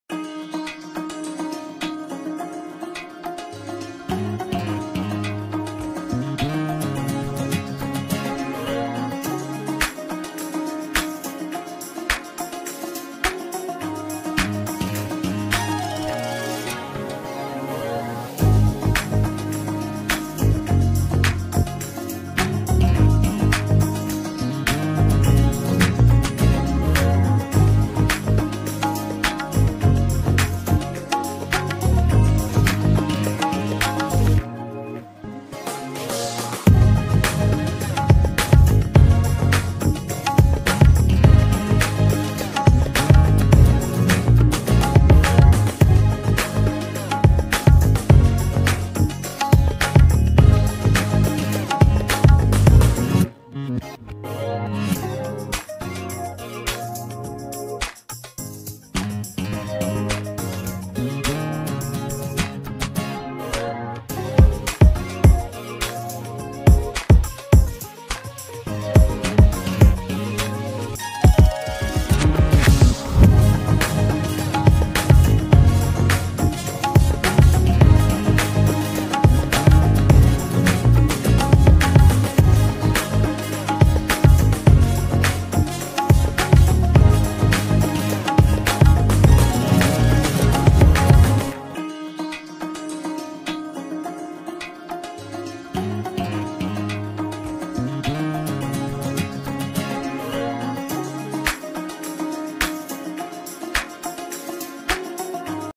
ONLINE TRP Report ll 34 Week 2023 ll Top 10 Shows ll @Telly One
ONLINE TRP REPORT OF ALL INDIAN SERIALS 34 WEEK 2023 UPDATES